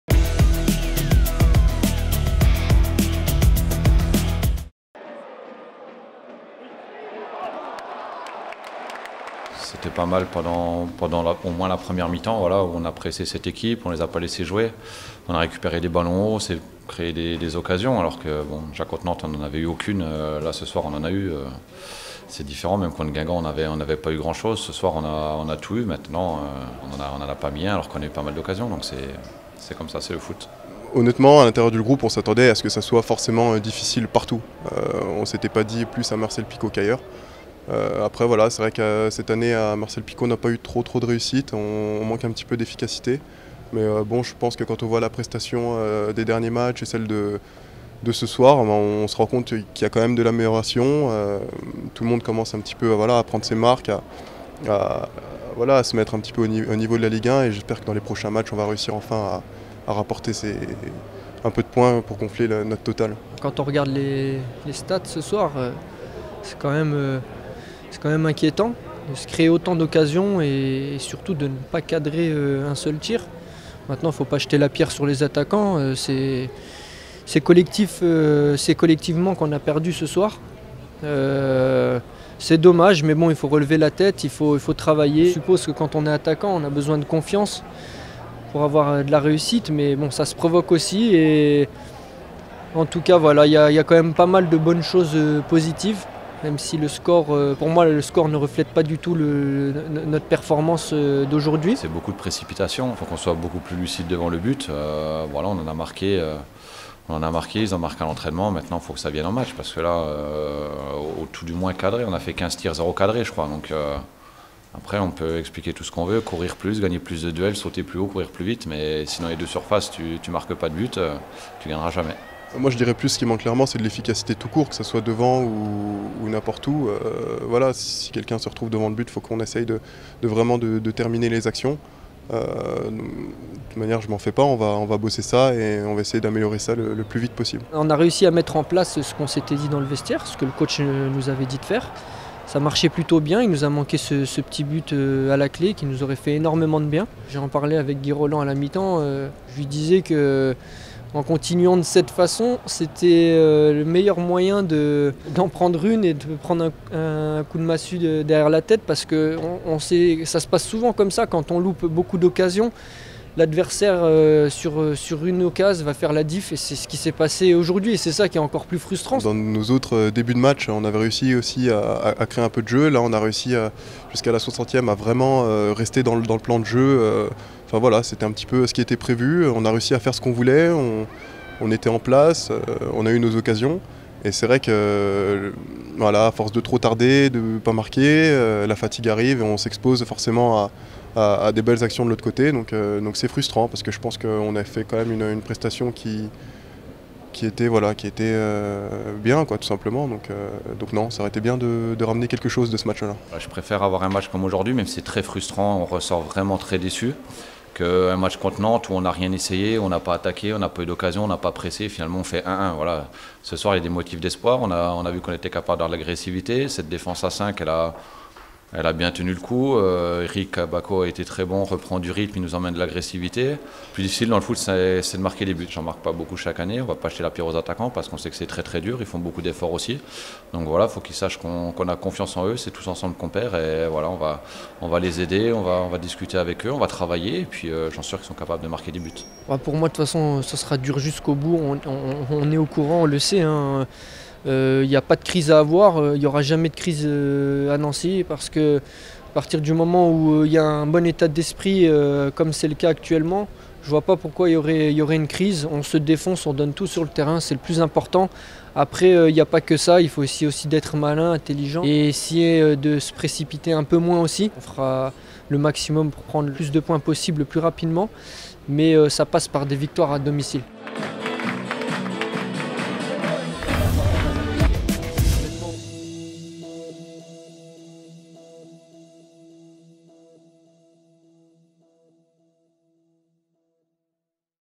Le décrassage d'ASNL-OGCN
Les réactions de Badila, Chrétien et Pedretti.